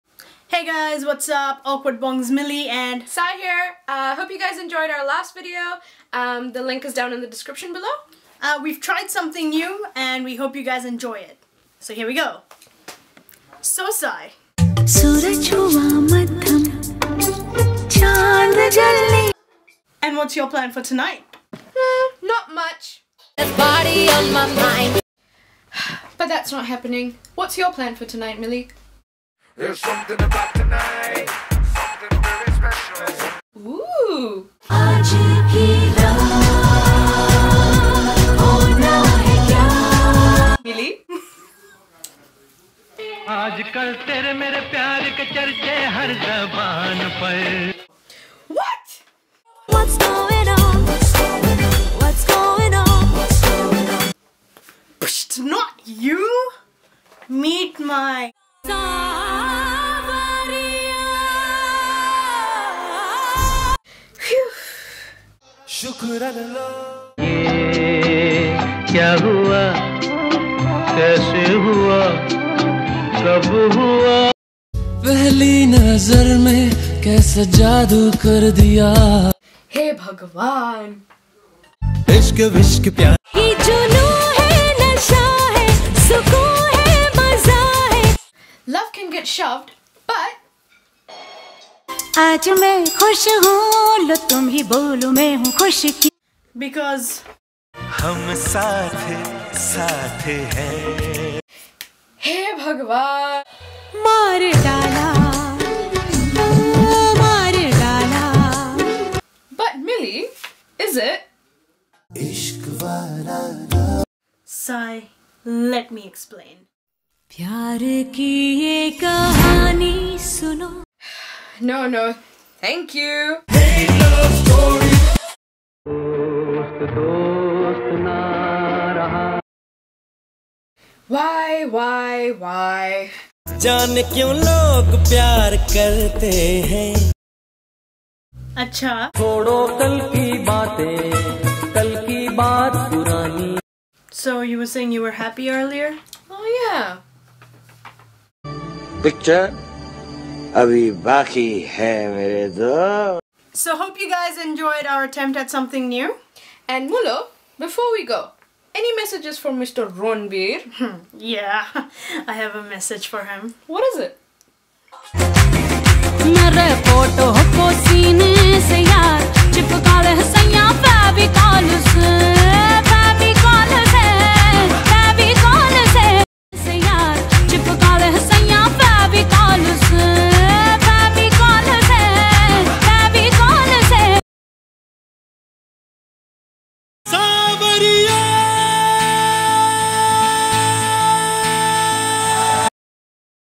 Ranbir and Mili's Love Story Bollywood Style
This is our 4th vlog and another attempt at something new!! Its a little 'all over the show' BUT its our first try!